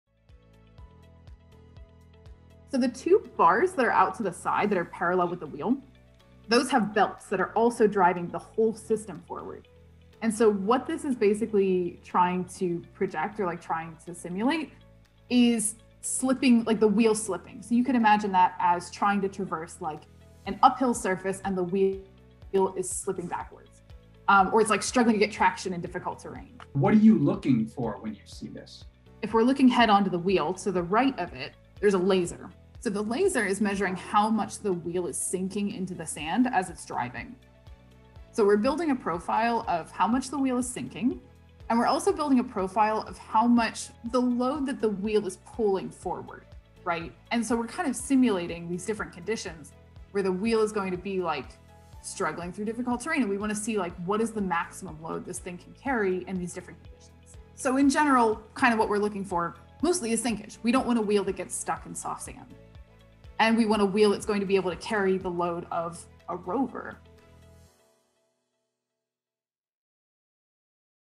Testing the Wheels for the Mars Perseverance Rover
A team of Carnegie Mellon University students and researchers assisted NASA's Jet Propulsion Lab with testing and designing the wheels of the Mars Perseverance Rover. CMU graduate Molly Whittaker explains the test, which was done on campus from 2015-16.